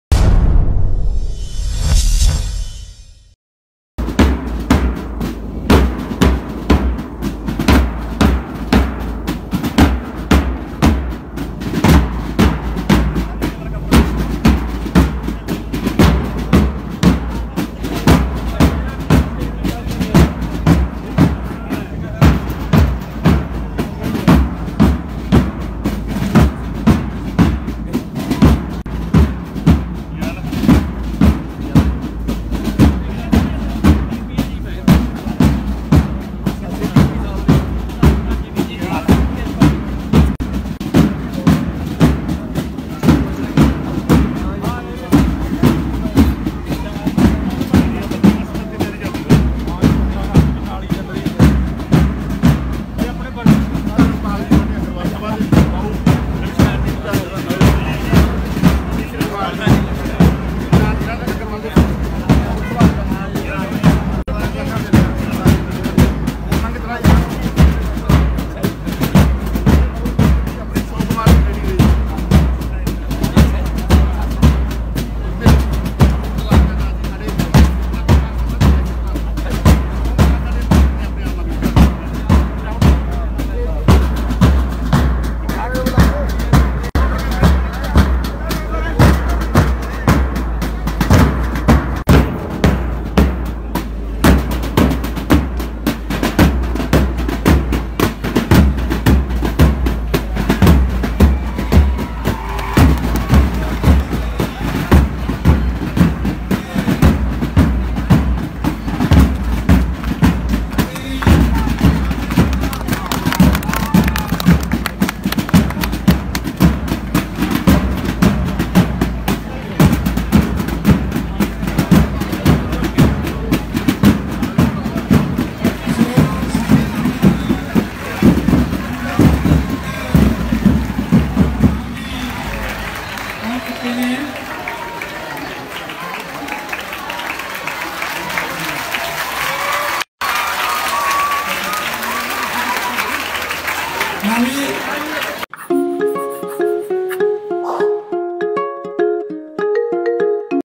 ਭਗਵੰਤ ਮਾਨ ਦਾ ਬੈਂਡ ਵਾਜਿਆਂ ਨਾਲ ਹੋਇਆ ਸਵਾਗਤ 😊
ਦਿੜ੍ਹਬਾ ਪਹੁੰਚਣ ਤੇ ਲੋਕਾਂ ਵਲੋਂ ਭਗਵੰਤ ਮਾਨ ਦਾ ਕੀਤਾ ਭਰਵਾਂ ਸਵਾਗਤ